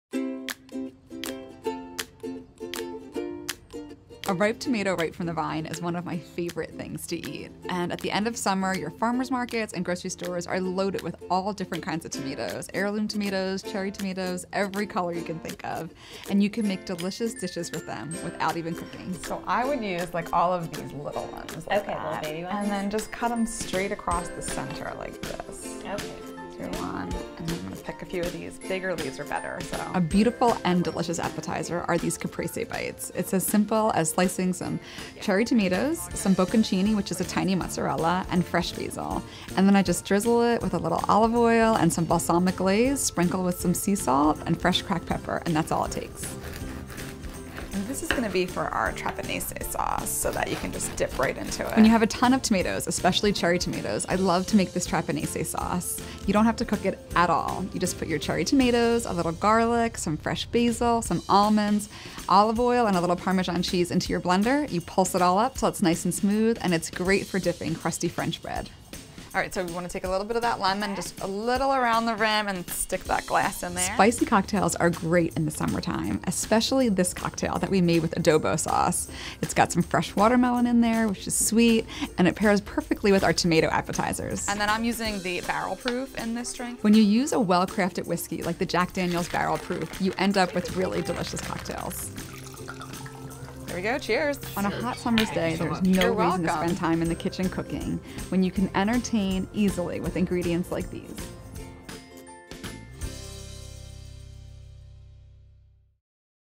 No Cook Entertaining- Tons of Tomatoes & Spicy Cocktails
If you have a bumper crop of cherry tomatoes this is a must watch! Easy no cook appetizer ideas -a sauce inspired by Lidia Bastianich but with my own twist and a spicy cocktail perfect for clinking with friends!